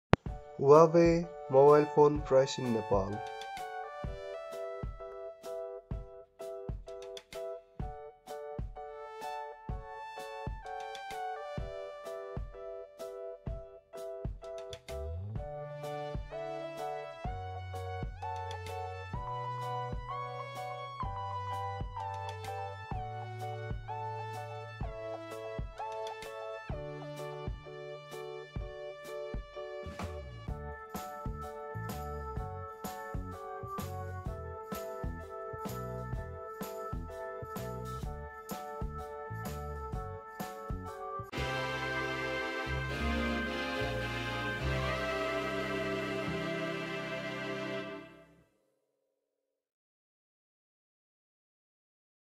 Huawei Mobile Price in Nepal | Huawei Phones Prices in Nepal - 2019
Here is Huawei mobile price in Nepal as updated on June 2019. Please note that this may not be complete price list of Huawei mobile phones available in Nepal, which is subjected to change with time. For most recent details, please visit your nearby distributors or retailers.

This video covers price and specifications of most of the following Huawei mobile phones in Nepal:

- Huawei P30
- Huawei P20
- Huawei P20 lite
- Huawei P Smart 2019
- Huawei Nova 4
- Huawei Nova 4e
- Huawei Nova 3
- Huawei Nova 3e
- Huawei Nova 2 plus
- Huawei Nova 2 lite
- Huawei Mate 20 (Porsche Design)
- Huawei Mate 20 X
- Huawei Mate 10 (Porsche Design)
- Huawei Y Max
- Huawei Y9 Prime 2019
- Huawei Y9 2019
- Huawei Y7 Prime
- Huawei Y7 Prime 2018
- Huawei Y7 Pro 2019
- Huawei Y6 Prime 2019
- Huawei Y6 Prime 2018
- Huawei Y6 Pro 2019
- Huawei Y5 Prime 2019
- Huawei Y5 Prime 2018
- Huawei Y5 2019
- Huawei Y5 Lite